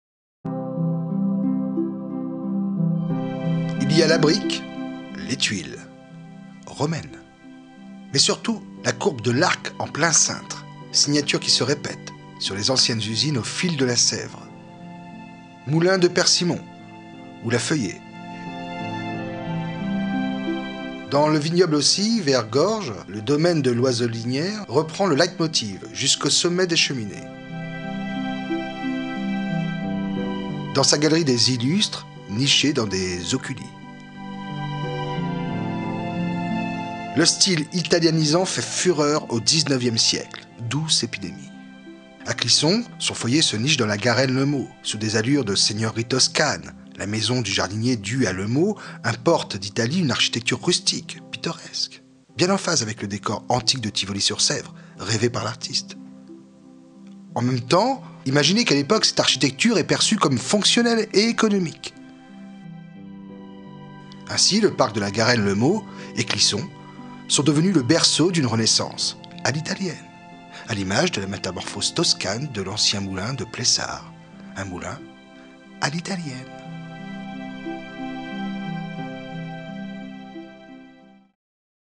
Découvrez l'architecture italienne de Clisson !
Avec Mobil'iti, découvrez avec un iPhone, sur les rives de la Sèvre à Clisson, une promenade insolite parmi d'anciennes filatures et d'anciens moulins à farine ou à papier, bâtis dans un style italianisant. Un patrimoine sans équivalent.

Sur la rive gauche, vous allez découvrir le moulin de Plessard. A la vue de ses tours, ses baies géminées, ses arcs et claustras, difficile d'imaginer qu'il a pu héberger une quelconque activité industrielle. Ce qui fut pourtant le cas.
Le site délicieux qui abrite Plessard fait face au parc romantique de la Garenne-Lemot. Son promoteur, Frédéric Lemot, soucieux de ne pas voir la vue gâchée, l'a acquis en 1821. On doit la construction d'un second moulin à son fils, Barthélémy Lemot. Le bâtiment a reçu en 1901 le corps supplémentaire qui parachève son caractère éminemment pittoresque.
Il abrite désormais une base de canoë-kayak.
Plus loin le long de la rive, vous rencontrerez d'autres surprises : la Feuillée, dont l'immense cheminée semble surgir de l'eau à travers les frondaisons ; Persimon, en grès rose percée d'une baie unique ; l'Arsenal enfin, une ancienne filature noyée dans les saules pleureurs.
En direction opposée, c'est-à-dire vers l'aval, ne manquez pas de pousser jusqu'au site du Liveau, dont la façade évoque une vaste loggia. Superbe.

Extrait de Mobil'iti, guide de visite multimédia réalisé par Latitude Gallimard pour la Communauté de Communes de la Vallée de Clisson.

Vous pouvez le télécharger gratuitement pour votre iPhone ou iPod touch en suivant le lien :

Des appareils sont également disponibles sur place en location à l'Office de tourisme de Clisson.